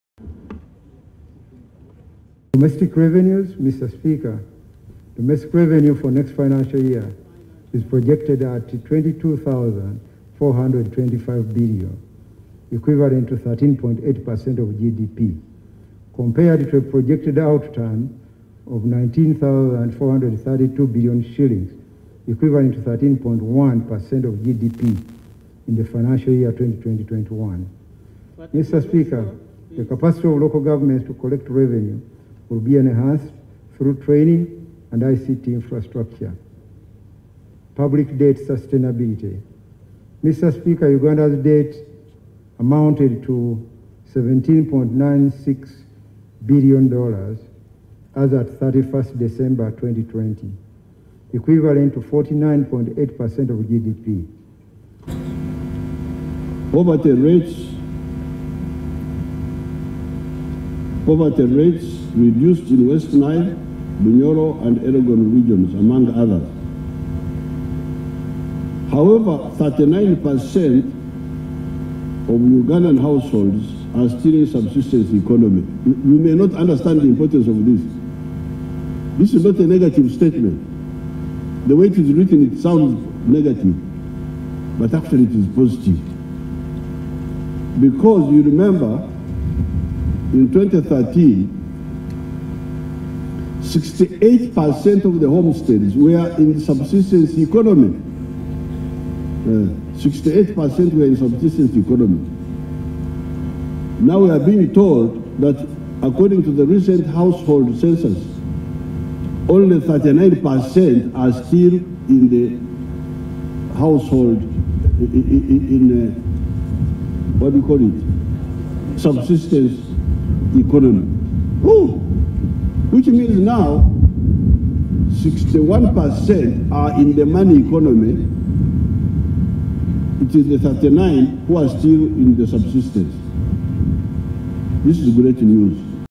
Embalirira ya 2021/22 esomeddwa olwaleero ya buwumbi 44.7
Embalirira y'omwaka 2021/2022 esomeddwa olwaleero ng'eno esuubirwa okuyamba bannayuganda okweggya mu bwavu. Embalirira eno ya buwumbi emitwalo ena mu enkumi nnya mu lusanvu era ng'esomeddwa omubaka wa Ntenjeru North Amos Lugoloobi ku lw'Omukulembez w'eggwanga.